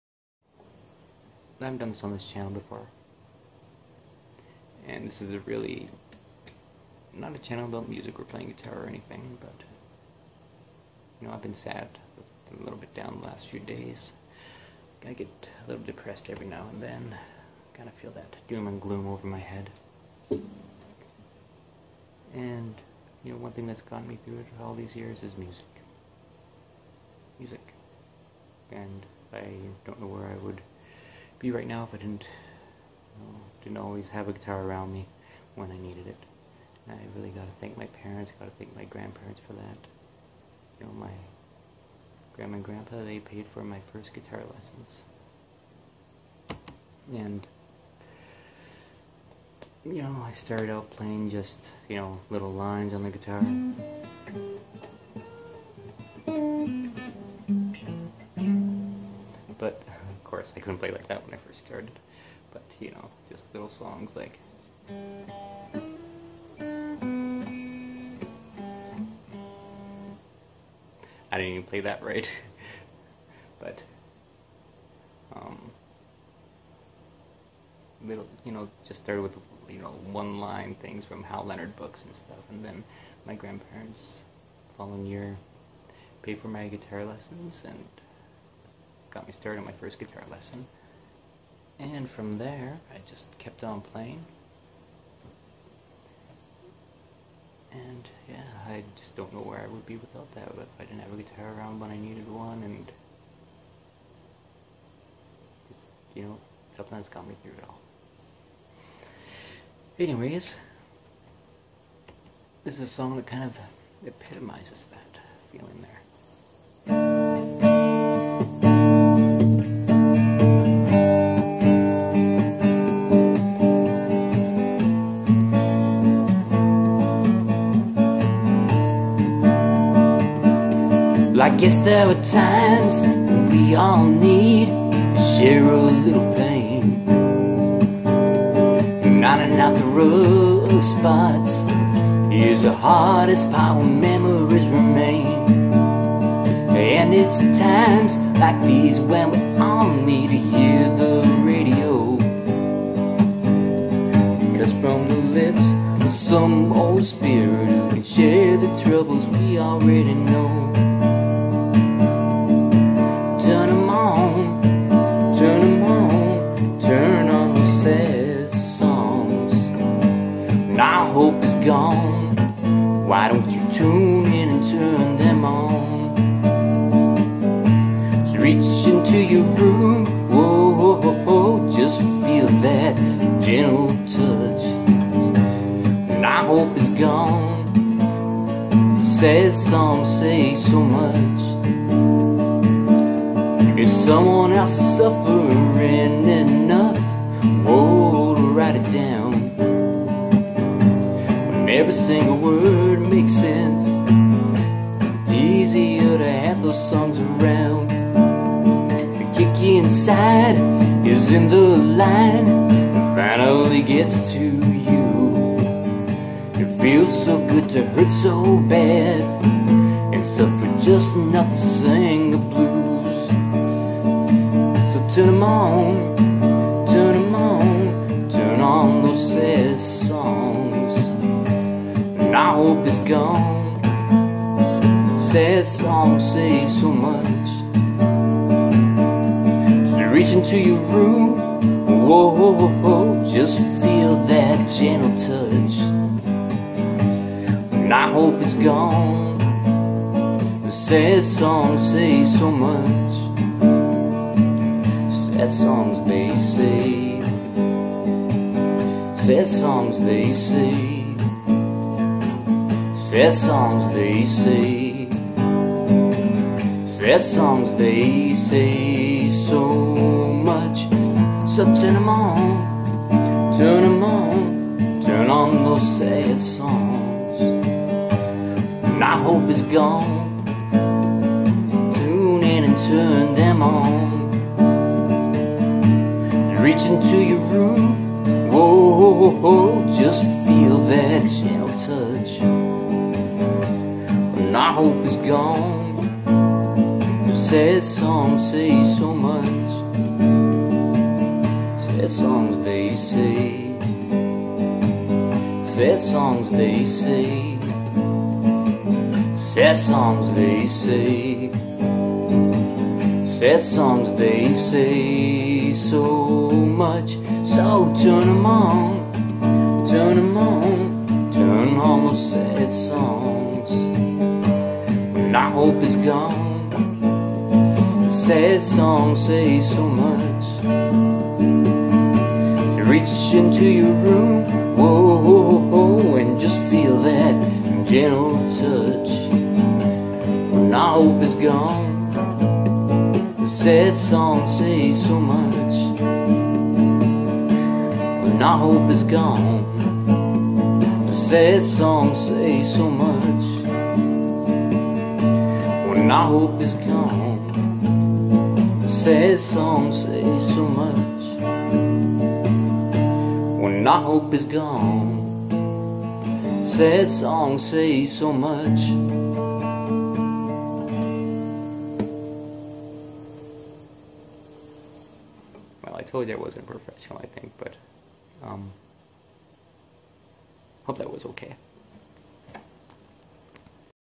Music Heals My Spirit
Music Heals my Spirit. I couldn't think of a better way to put it. Musical therapy has certainly been a big part of my life. I've been playing guitar since I was 12, almost 30 years. My grandparents payed for my first month of guitar lessons after I had taught myself how to pick a bit. When I'm down, I turn to music to heal me. Music truly does have a healing factor to it leading tme to belive in the validity of Music Therapy. While this channel isn't about music, I do feel it is abut the spirit. For me, there is nothing more soulful than singing and playing a song that lifts me up. I am by no means a professional and hope that my posting this does not put off any of my subscribers or future subscribers.  I do ocassionally break into song as you may have noticed in a couple of my videos, it's just part of who I am. But then it seems that spirits do the same thing so it can't be all that bad.